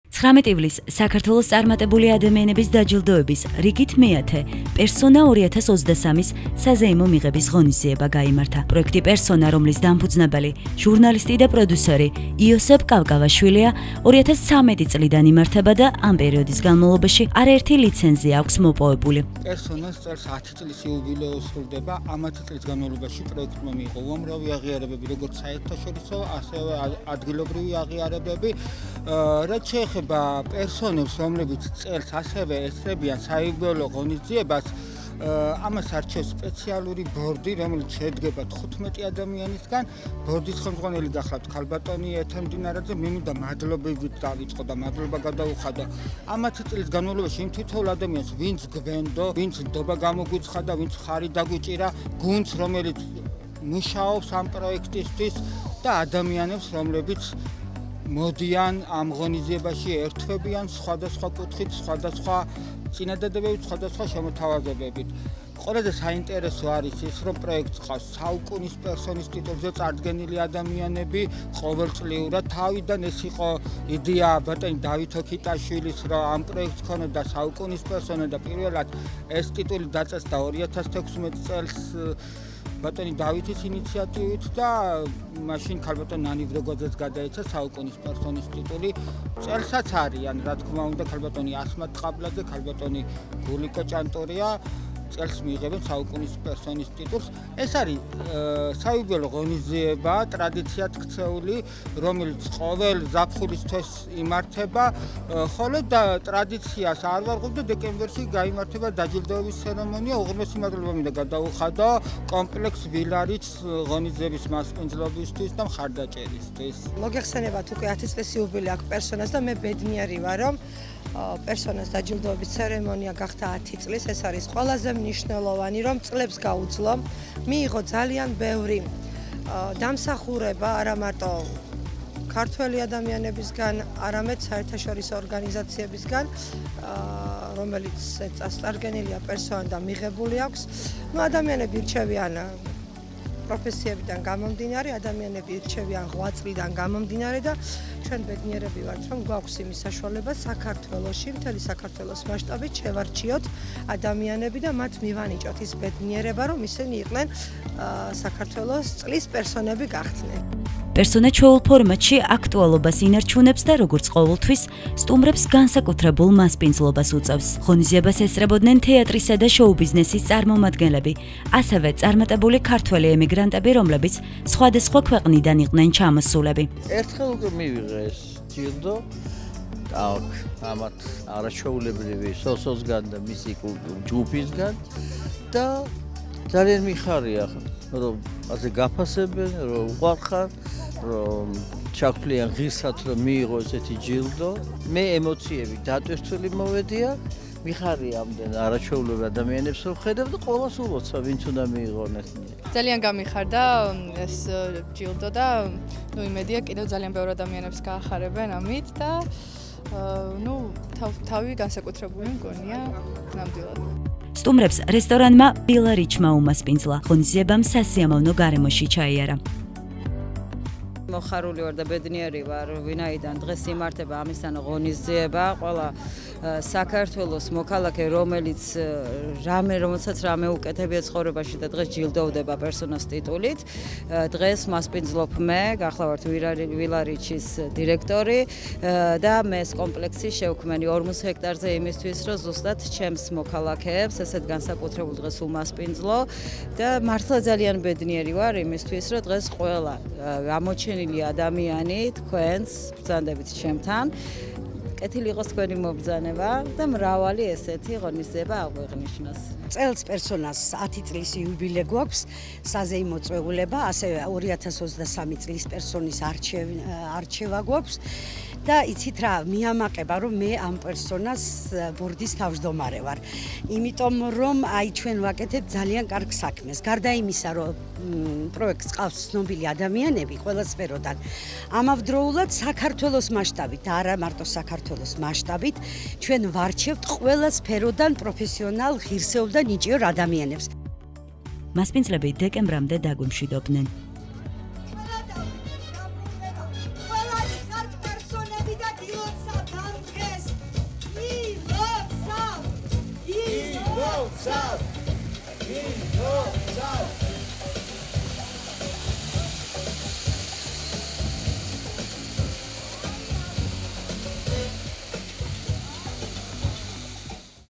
19 ივლისს საქართველოს წარმატებული ადამიანების დაჯილდოების ცერემონია, "პერსონა 2023" გაიმართა
19 ივლისს საქართველოს წარმატებული ადამიანების დაჯილდოების ცერემონია, "პერსონა 2023" გაიმართა
✖ Music Box